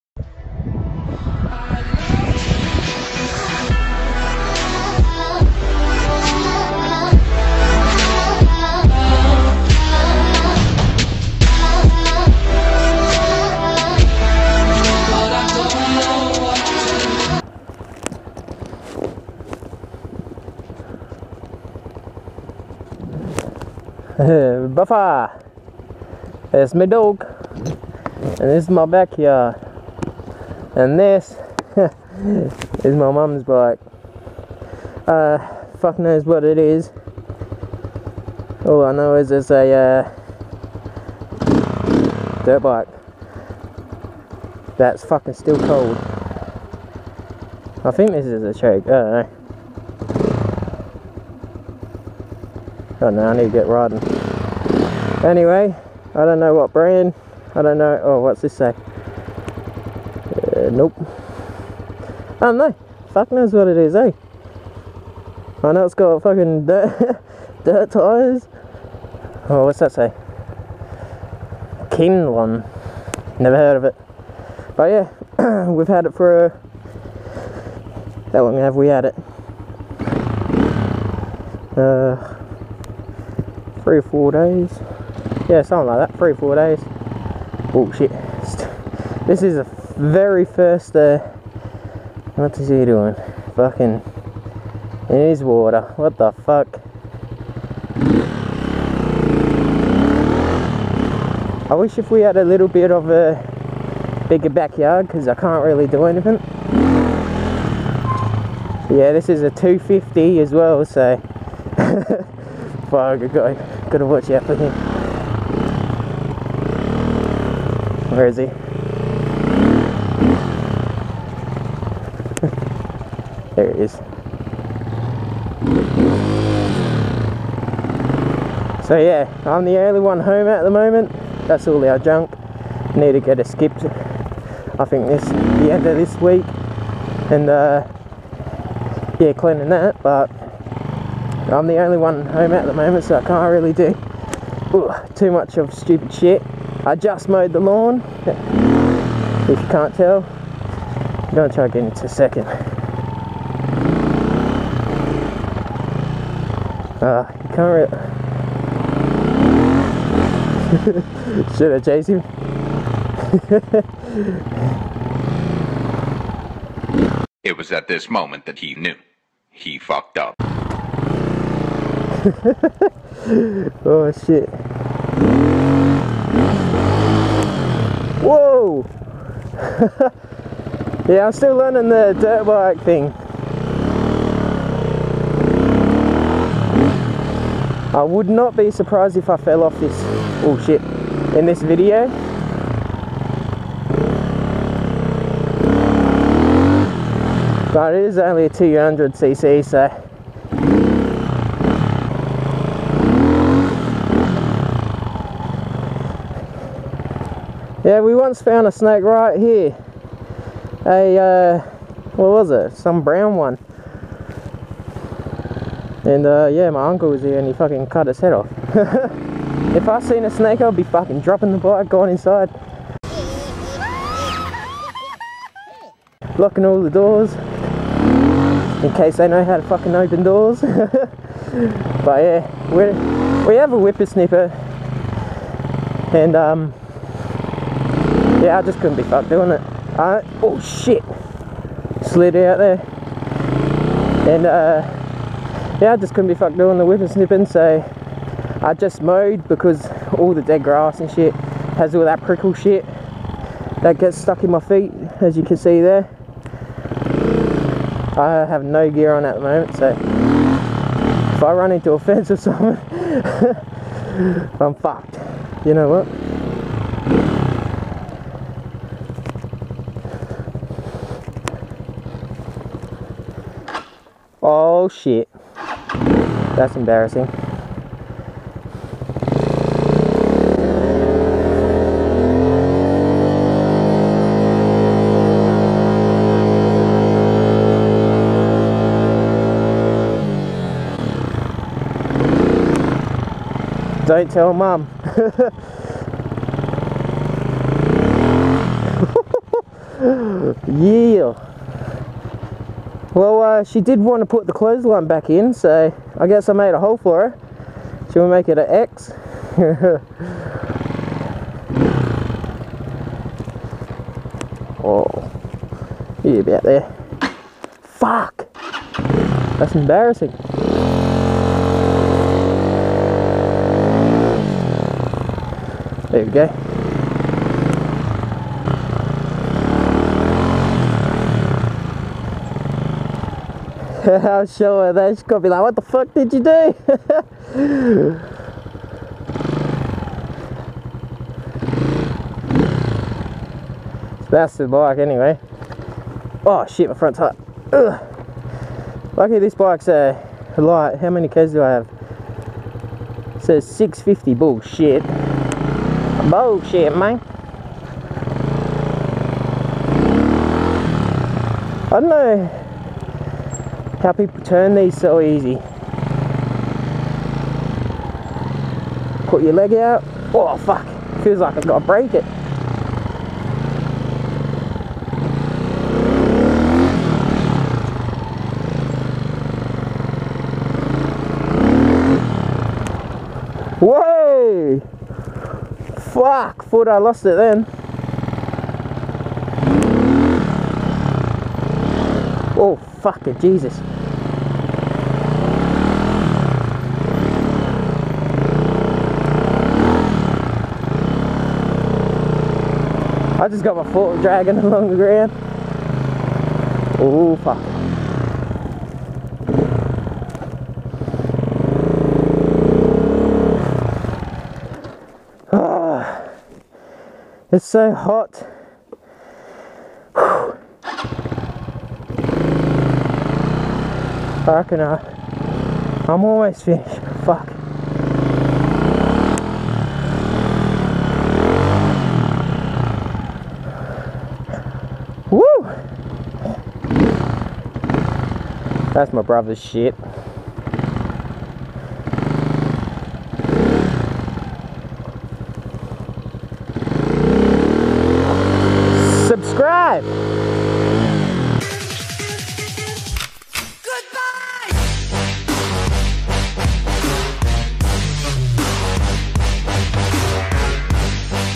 What I Do When Im Home Alone (BackYard MotoVlog)
So... i mowed the back lawn then got a little bored.. what else was i suppose to do? dig holes? well... i did that too but on a motorbike :D